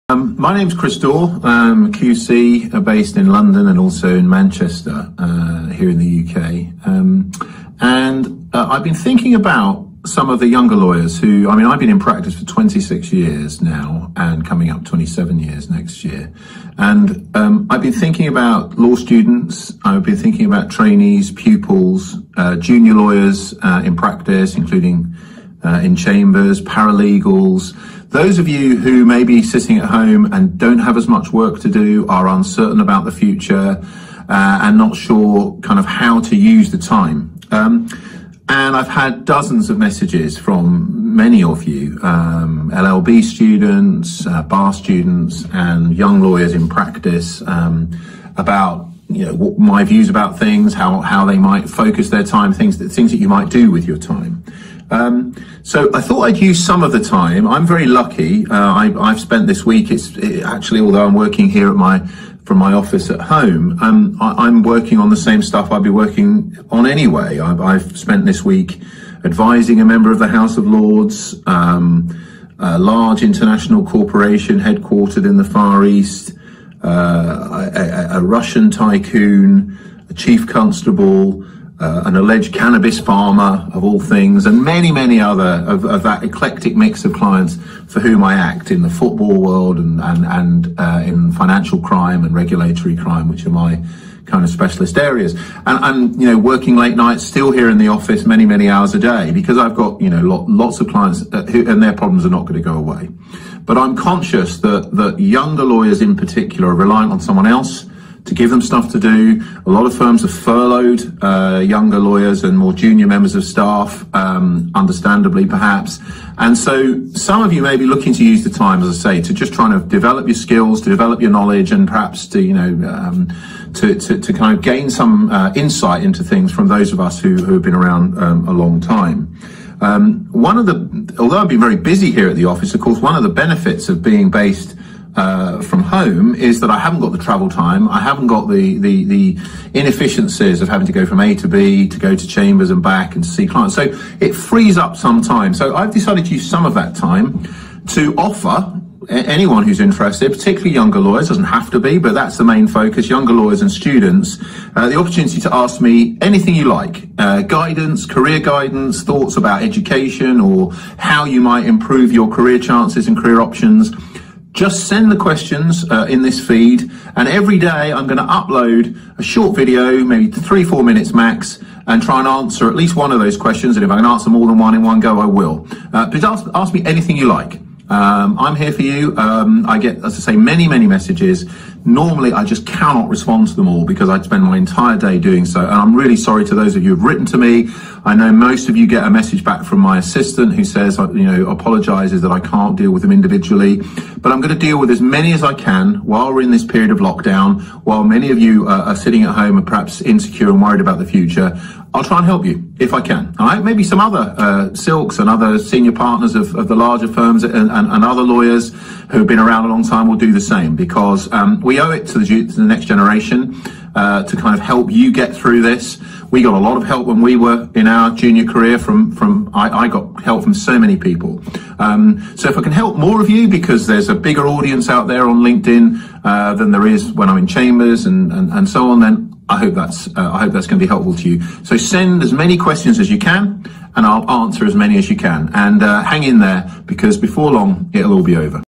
Ask Chris anything - LOCKDOWN Legal Mentoring - Day 1
In a series of videos filmed during the Coronavirus lockdown, Chris Daw QC offers daily advice and guidance to law students, trainees, pupils, juniors, paralegals and anyone interested in pursuing a career in law.  DAY 1.